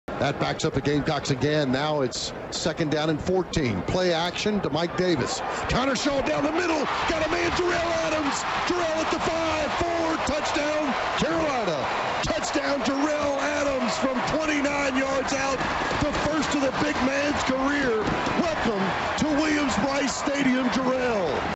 2012 USC vs Arkansas - Jerell Adams 29 Yd Touchdown Reception
Connor Shaw hits a wide open Jerell Adams for a touchdown.

Todd Ellis and Tommy Suggs on the call.